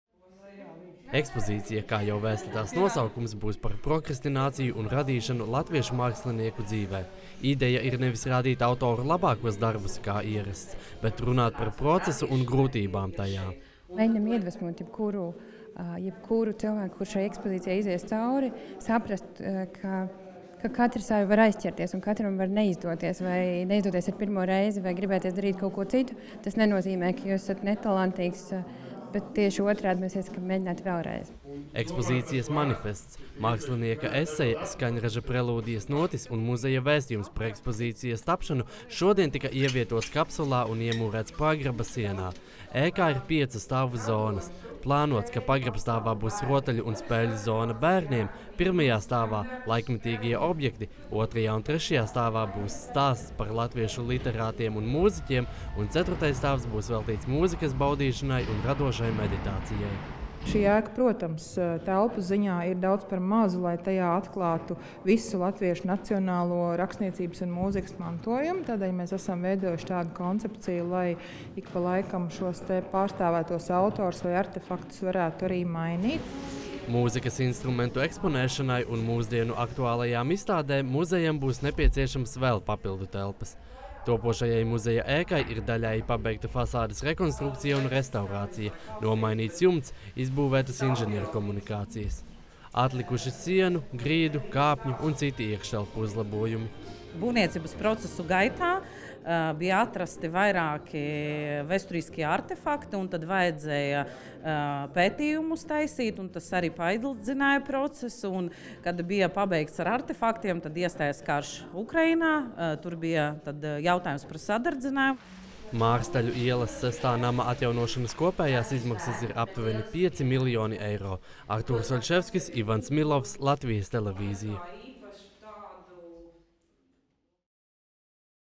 Iemūrēta kapsula Rakstniecības un mūzikas muzejā
Rakstniecības un mūzikas muzejs ilgu laiku bijis bez pastāvīgas ēkas. Taču šodien Vecrīgā, Mārstaļu ielas mājas pagrabā tika iemūrēta laika kapsula ar muzejā gaidāmās ekspozīcijas autoru darbiem. Muzeju plāno pabeigt nākamgad, taču apmeklētājiem tas durvis vērs 2025. gadā, kad būs muzeja simtgade.